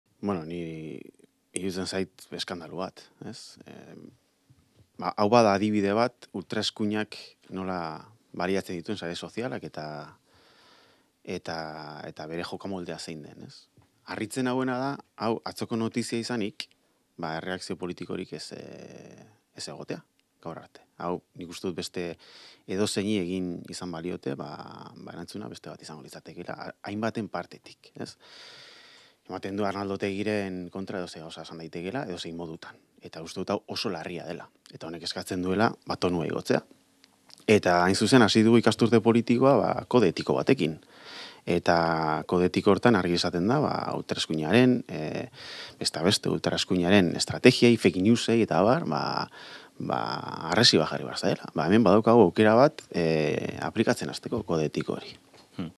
UPNk sareetan zabaldutako bideoaz galdetuta, Pello Otxandianoren erantzuna
«Ultraeskuinaren jokamoldearen adibide bat gehiago da. Harritu nau erreakzio politikorik egon ez izanak. Kode etikoaz hitz egiten ari garen honetan, aukera dugu halako jokamoldeei harresia jartzeko».